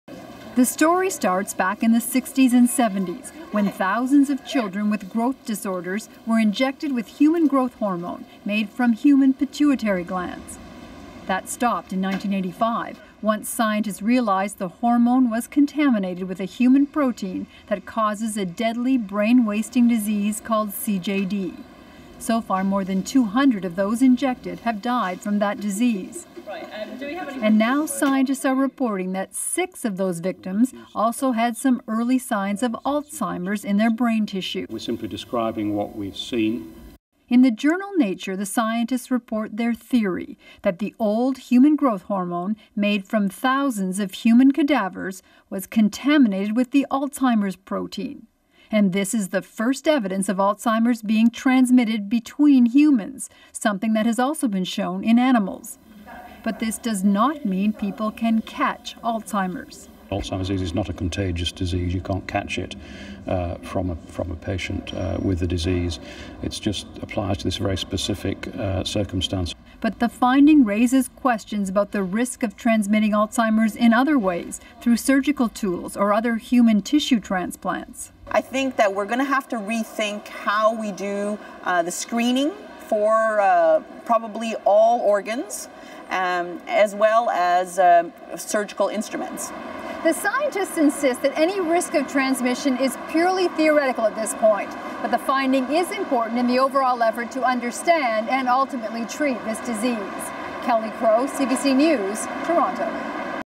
Alzheimer's traces found seeded in autopsied brains
An Alzheimer's protein has been found in the brains of people who received injections of human growth hormone.